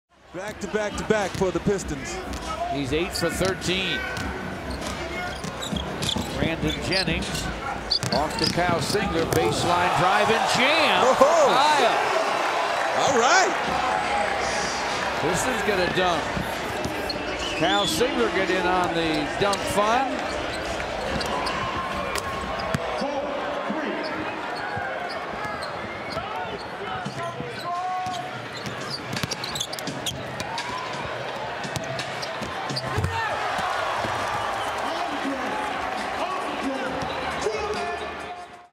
Kyle Singler Drives Baseline for the Two-Handed Jam on Kenneth Faried
Kyle Singler drives past Wilson Chandler and puts it home with power. for more highlights.